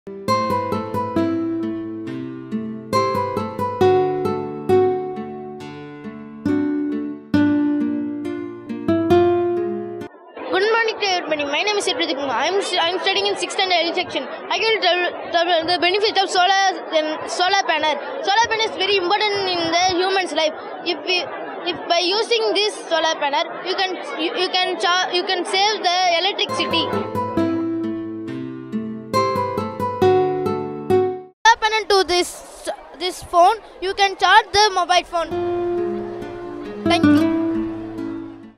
7. Charging the phone with Solar Energy
Amalorpavam Intra School Science Exhibition, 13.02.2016 ,  Saturday.